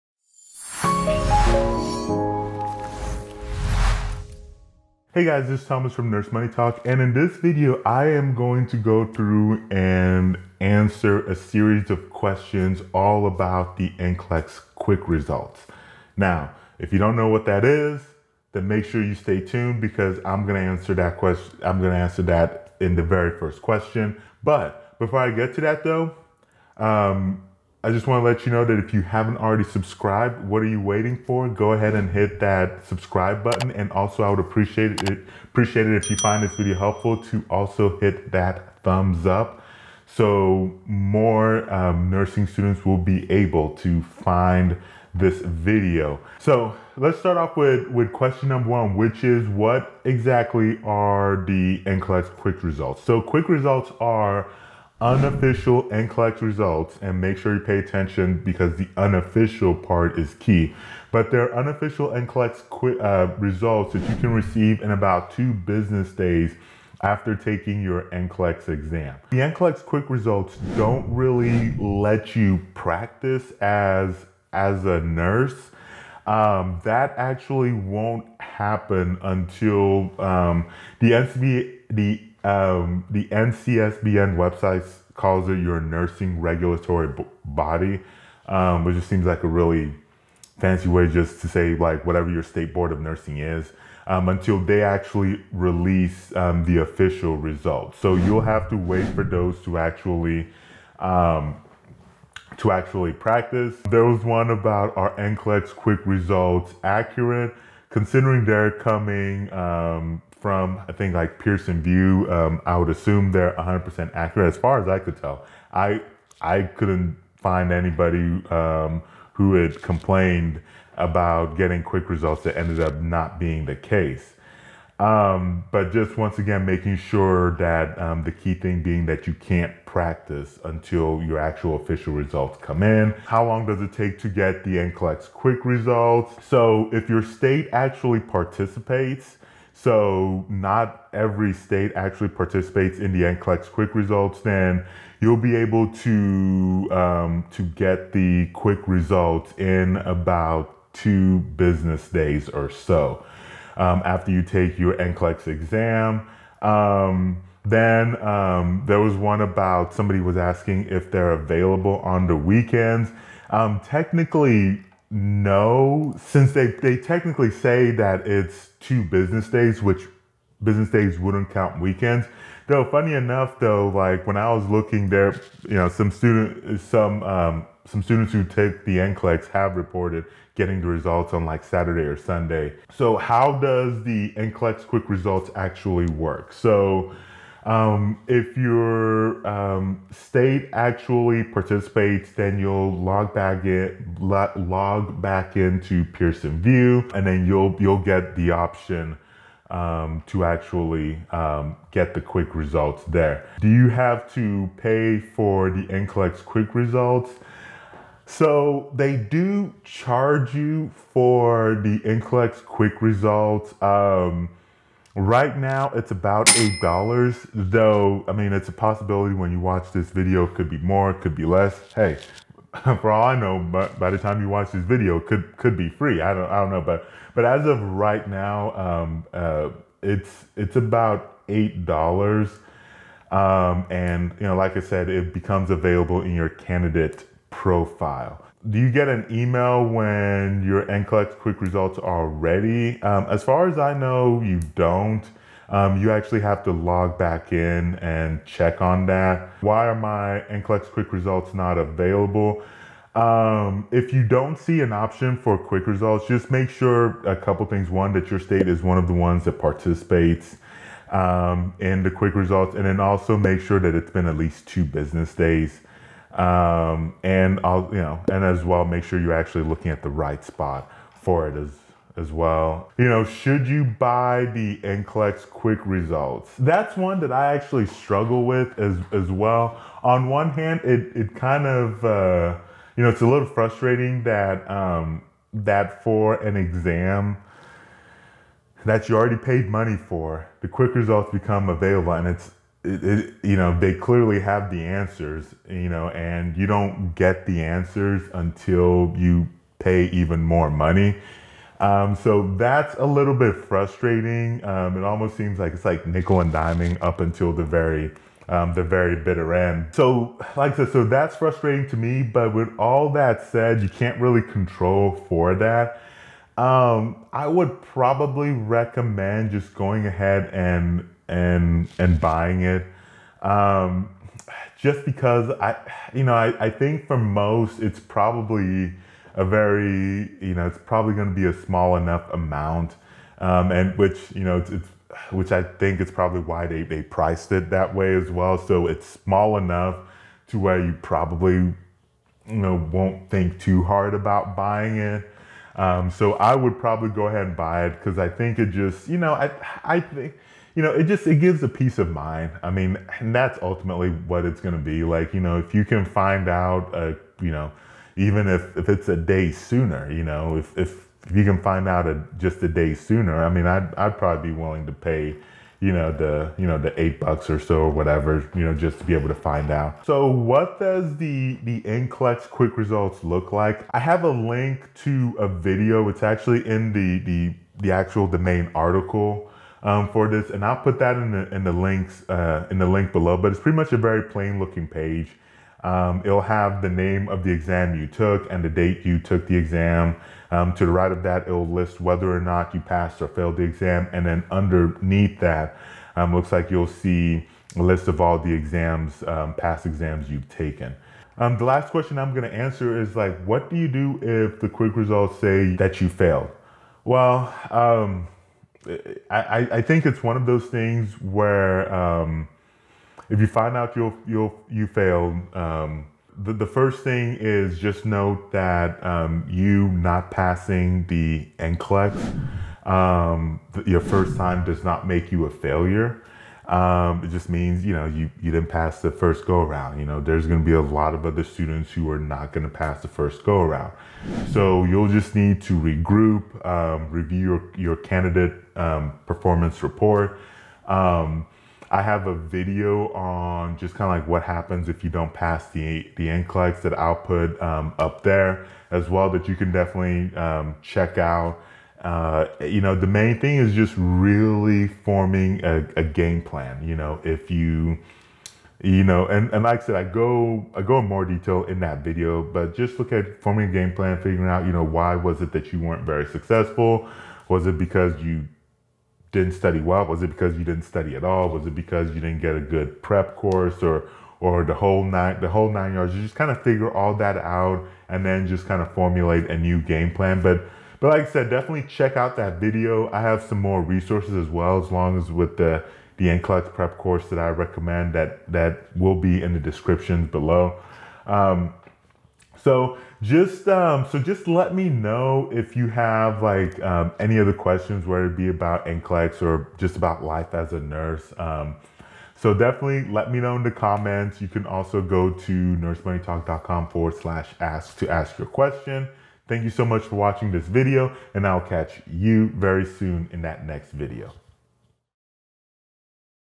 All About the NCLEX Quick Results | Things You Should Know!
Are you looking for answers about the NCLEX quick results? In this video, I'm going to answer some of the more frequently asked questions involving the NCLEX quick results.

➔ Nursing Student Academy (Get help on tough topics in nursing school)

Timestamps:
00:00 - All About the NCLEX Quick Results
00:46 - #1 What Is NCLEX Quick Results?
01:43 - #2 Are NCLEX Quick Results Accurate?
02:13 - #3 How Long Does it Take to Get the NCLEX Quick Results?
02:40 - #4 Are NCLEX Quick Results Available on the Weekends?
03:02 - #5 How Does the NCLEX Quick Results Work?
03:23 - #6 Do You Have to Pay for the NCLEX Quick Results?
03:58 - #7 Do You Get an Email When NCLEX Quick Results are Ready?
04:09 - #8 Why Are My NCLEX Quick Results Not Available?
04:23 - #9 Should You Buy the NCLEX Quick Results?
06:09 - #10 What Does NCLEX Quick Results Look Like?
06:40 - #11 What to Do if Quick Results Say I Failed?
08:05 - Let Me Know Your Thoughts!